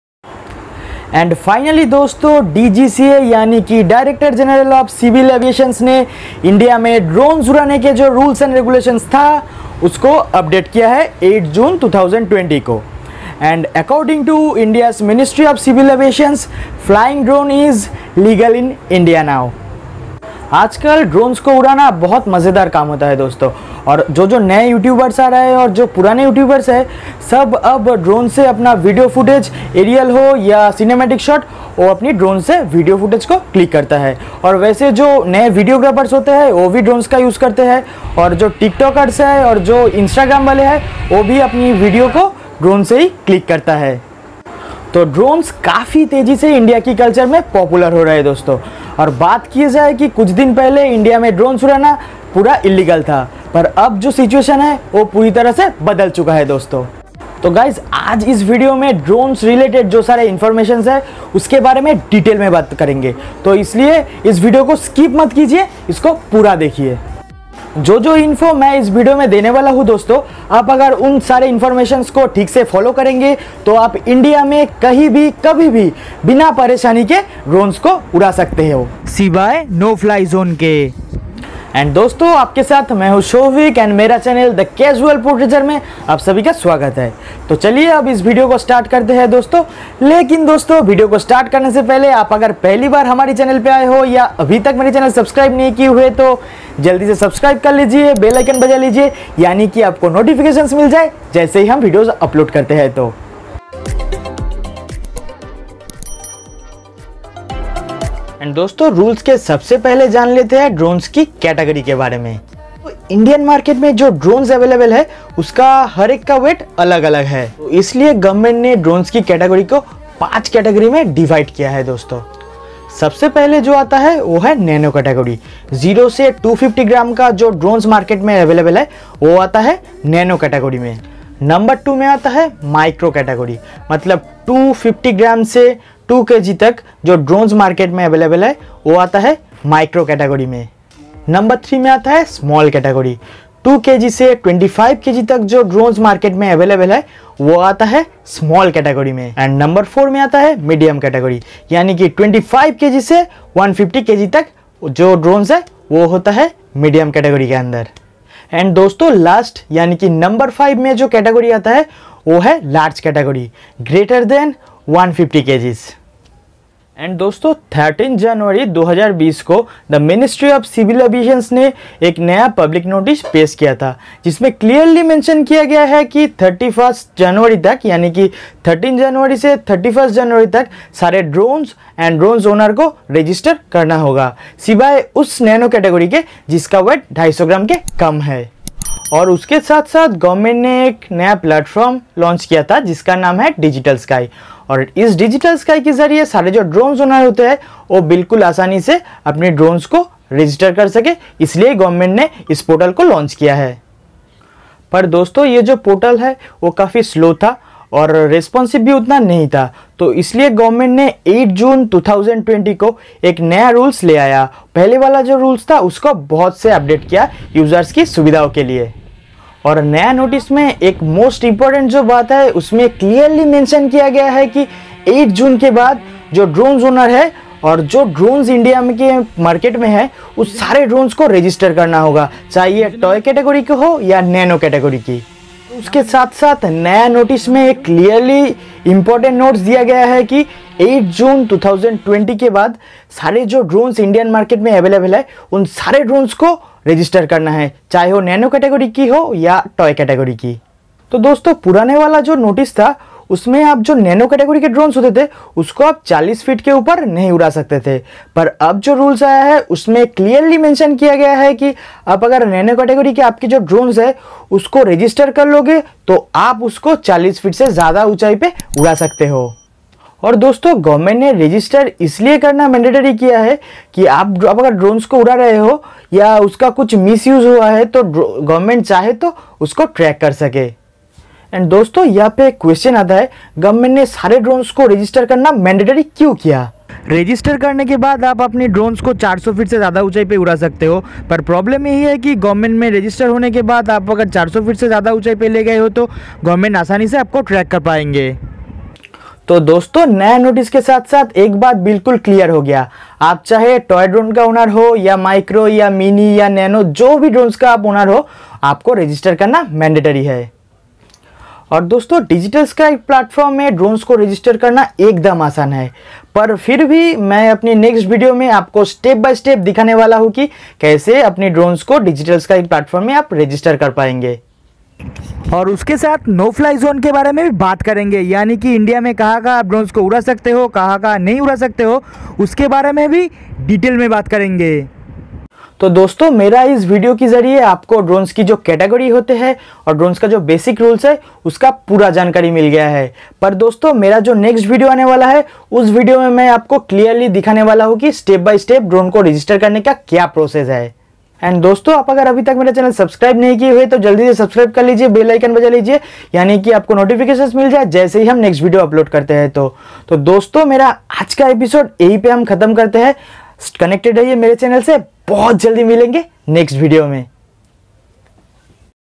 New Drone Laws by DGCA in INDIA on June 2020 | Things to know before flying and buying a Drone
tech talk #3
Drone Laws in INDIA 2020 Explained. New rules comes out june 2020. WATCH complete video to know all details. And contect to my channel.

🔸additional device : Realme XT
🔸Selfie Tripod - Gopro
🔸Location : Kolkata